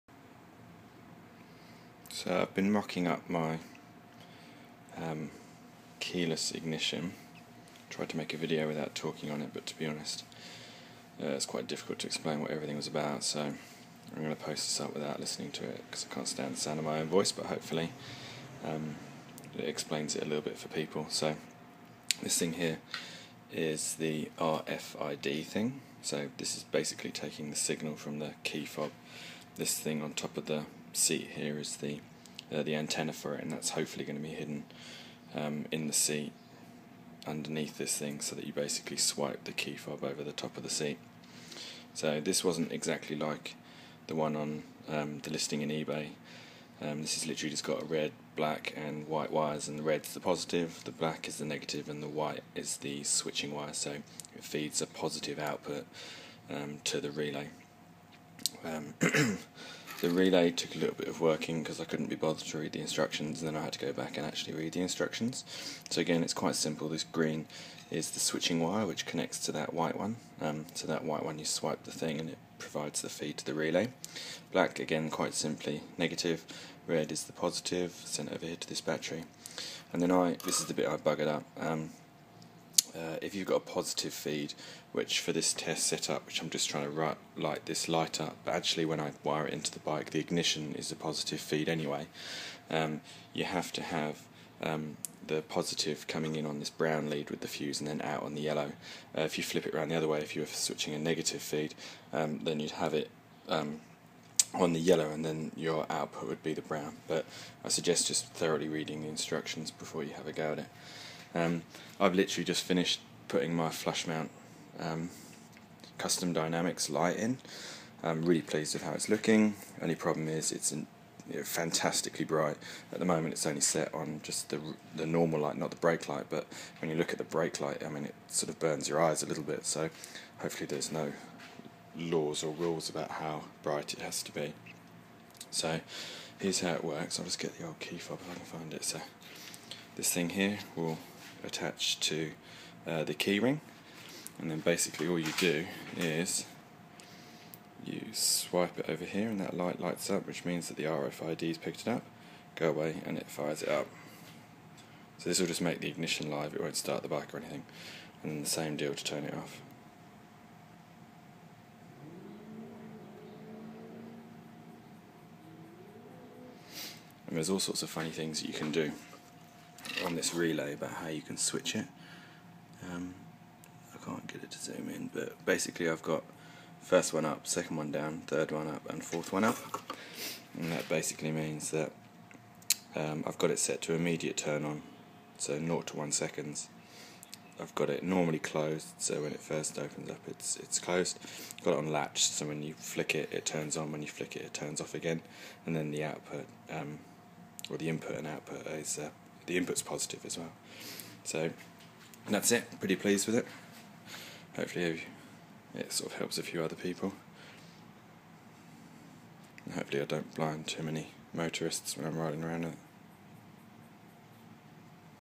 Wireless ignition test
Testing out the RFID and relay I bought to build my wireless ignition for my custom Honda CB400F cafe racer.

DEI 611T 12 VOLT 'ACTIVE
RFID Reader transponder key fob security